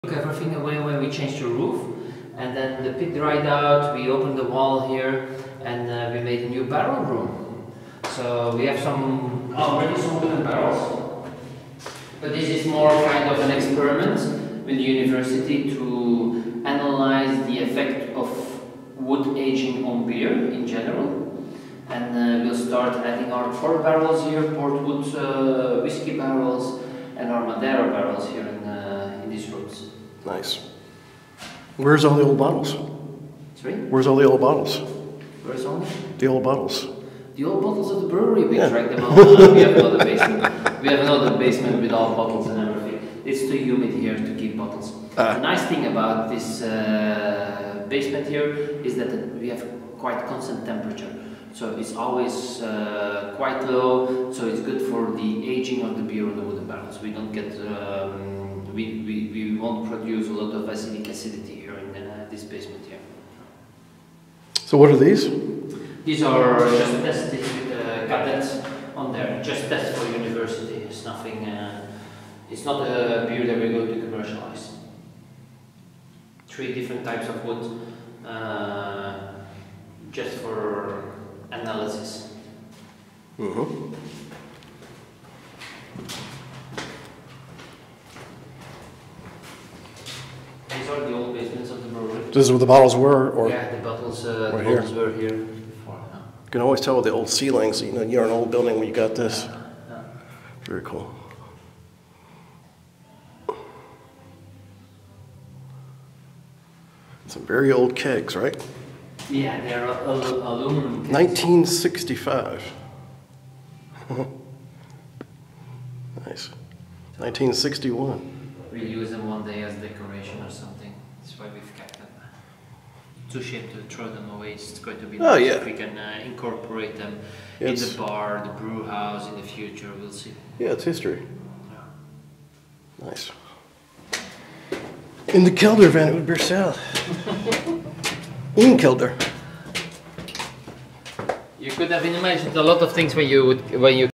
Gert Christiaens talks about some aging experiments in a cellar at Oud Beersel
Gert Christiaens talks about the cellars of Oud Beersel, where they now have some barrels full of various beers, doing some barrel-aging experiments. This cellar is where the vintage beers used to be stored before the brewery originally closed in 2002. There are also old kegs from the 1960's in the cellar, and more barrels will be added here in the future.